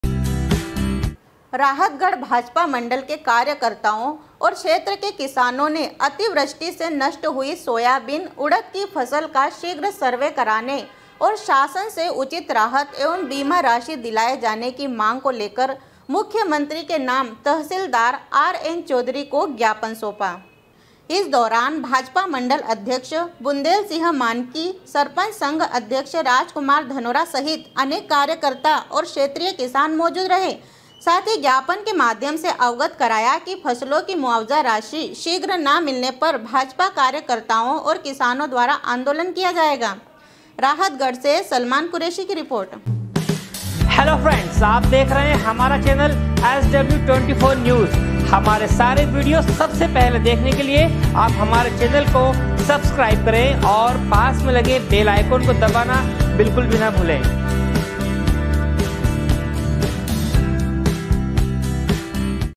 sw24news rahatgarh, 16.09.2019
राहतगढ़ भाजपा मंडल के कार्यकर्ताओं और क्षेत्र के किसानों ने अतिवृष्टि से नष्ट हुई#sw24news